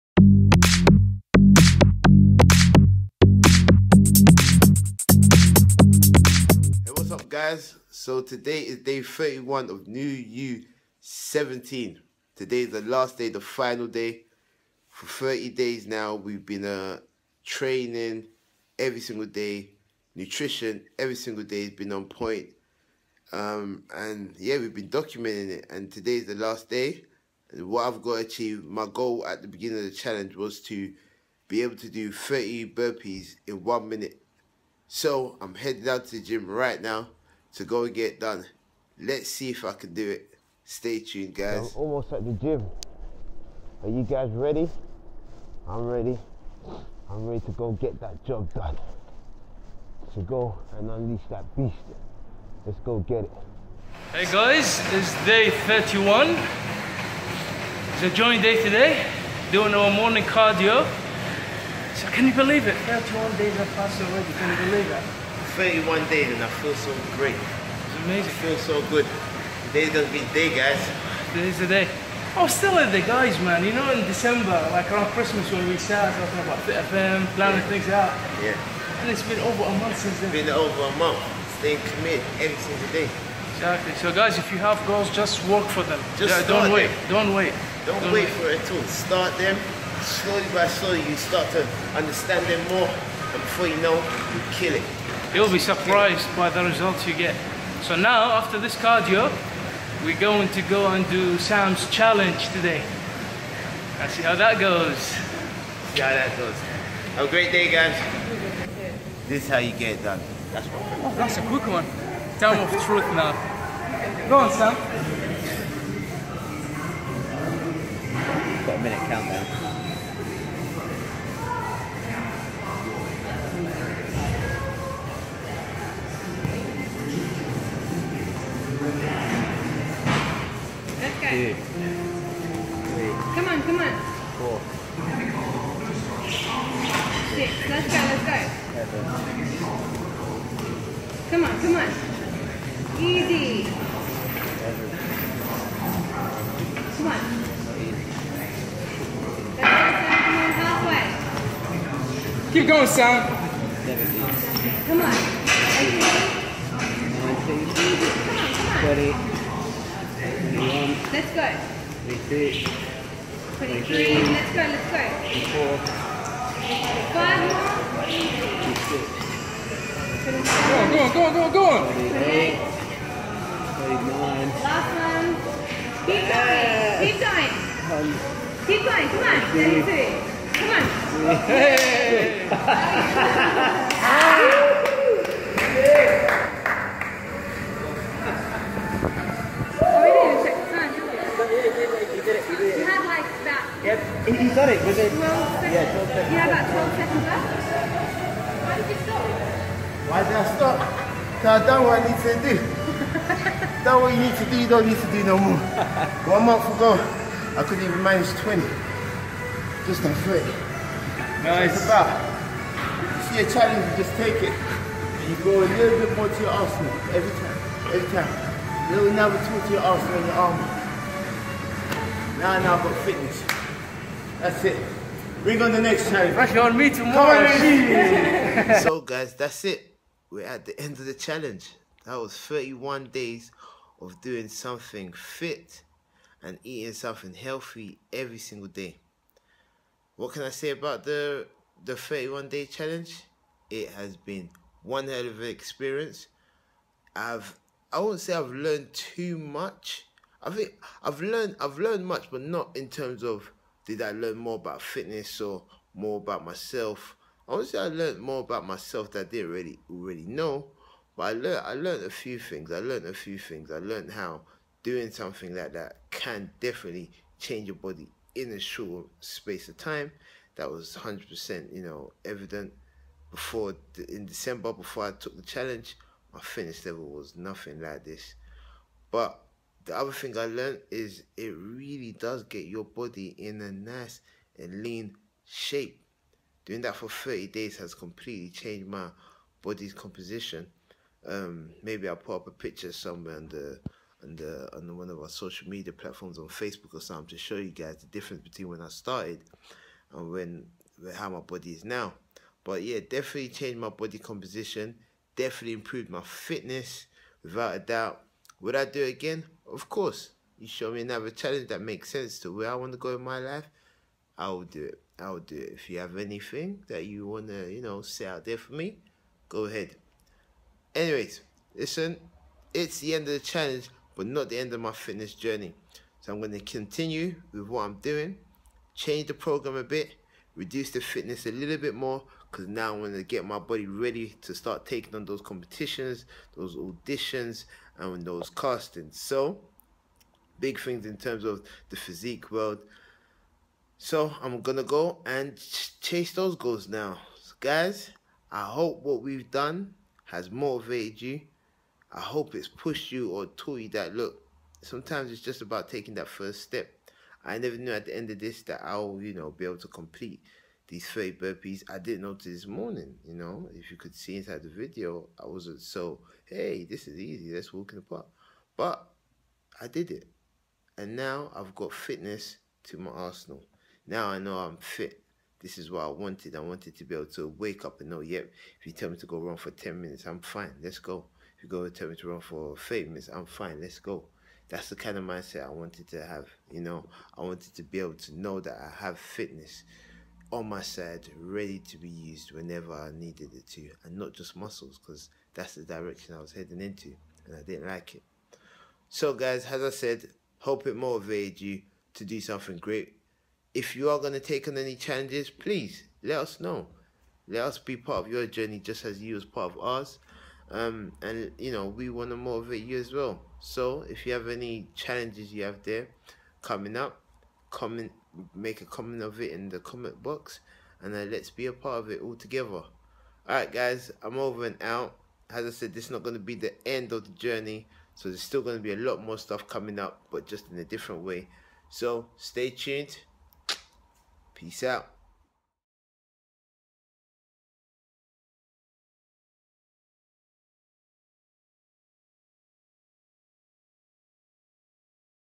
Newyou17 Day 31- How to get fit
So this is the end of the Newyou17 saga, am I a new person YES I have brought fitness into my life and it feels great. This was a big thing to me, having a good body without the fitness was never what I wanted for my body, but it was heading that way so this change was perfect for me. So I hope this was motivating for you and you will try something new this year go out there and get the job done.